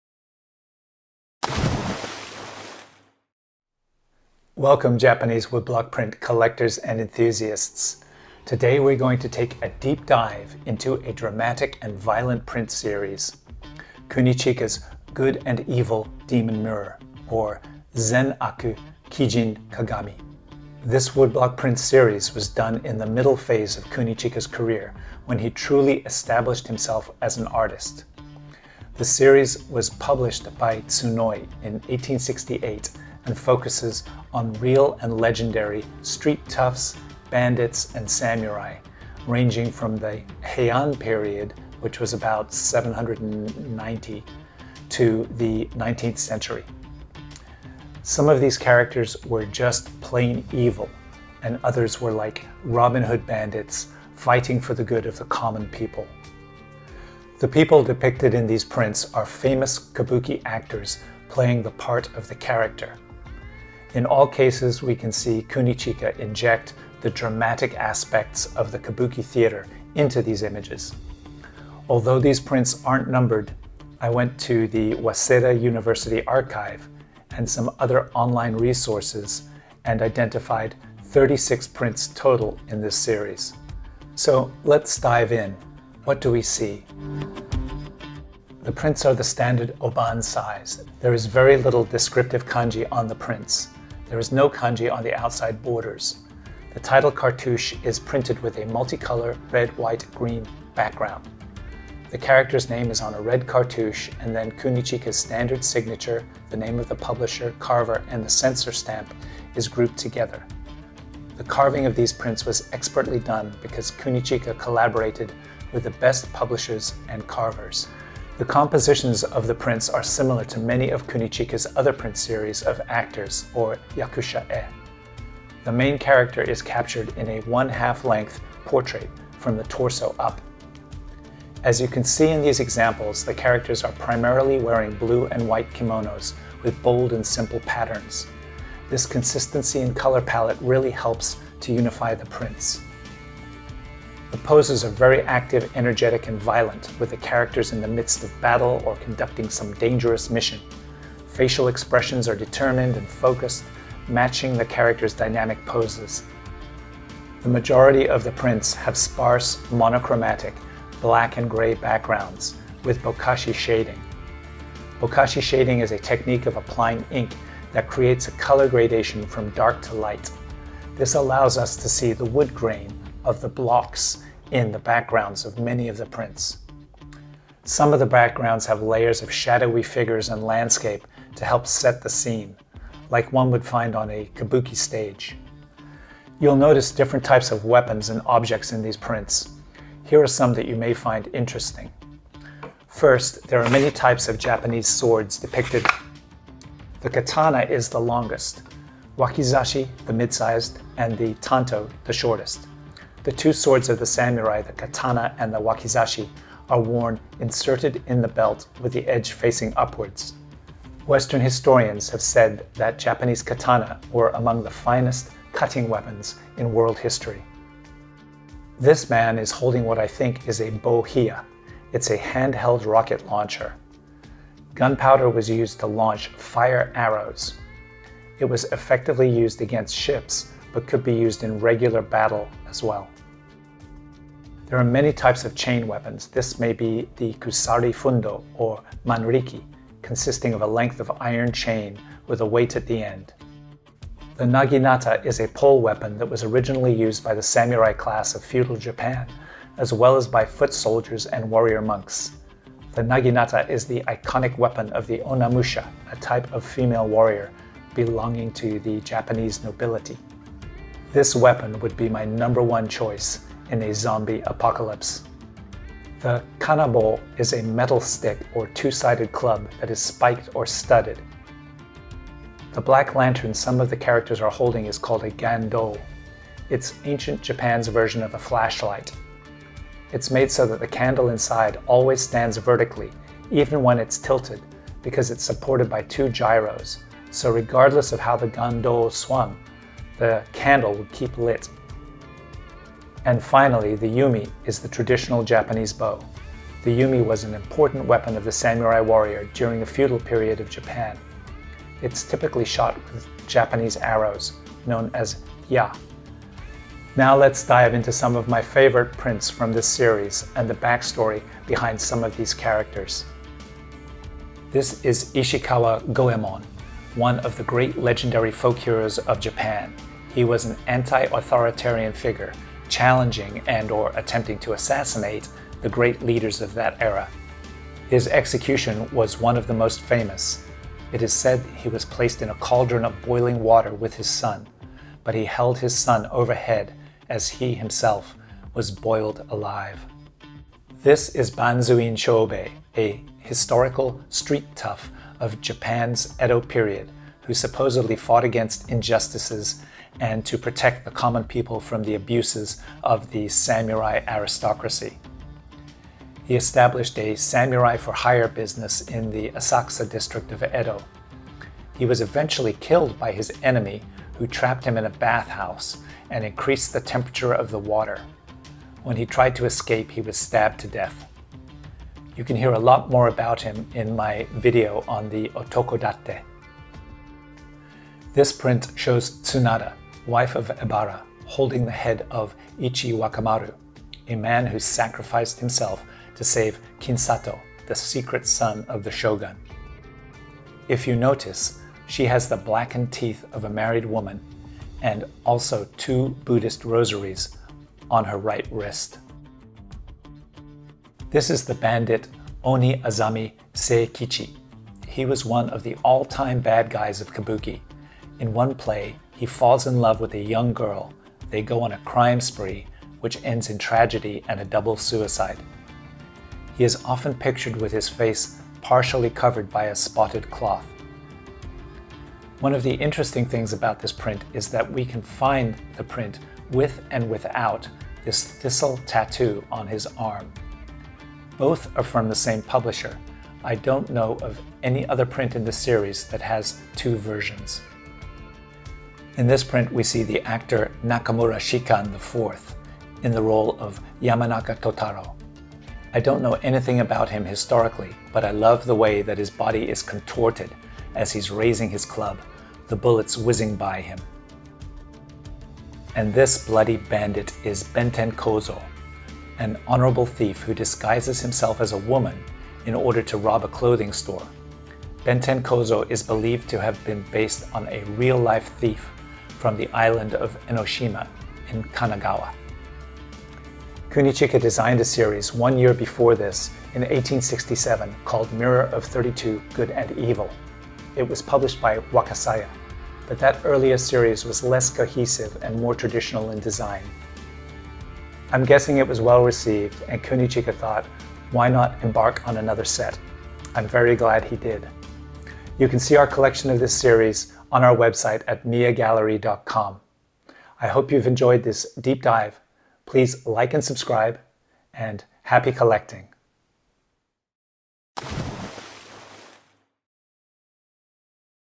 Deep Dive 3: Good and Evil Demon Mirror
This violent Japanese print series consists of bandits, street toughs and samurai. The name of the series is "The Good and Evil Demon Mirror" by Toyohara Kunichika, published in 1868. The series is held together, not only by the intense subject matter, but also by the consistency in pose and color palette. Join us as we dive into aspects of the ancient weapons, and stories of the characters behind these prints.